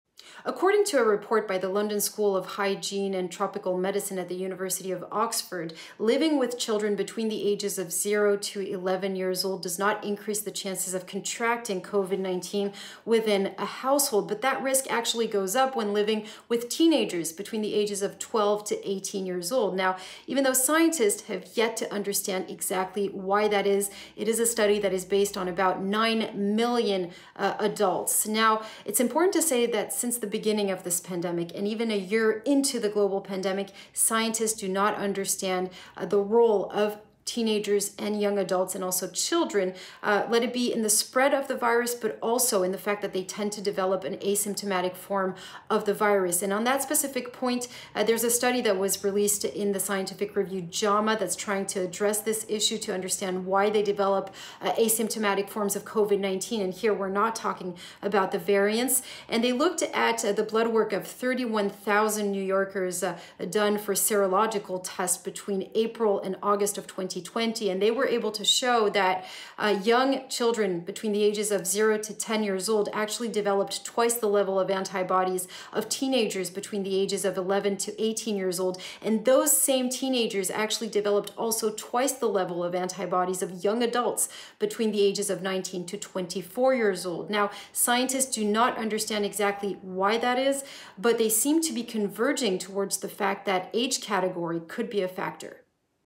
Children have stronger antibody response to Covid-19, study says
Children who are 10 and younger produce more antibodies in response to coronavirus infection than adolescents and adults, a study showed Monday. FRANCE 24's Julia Sieger tells us more.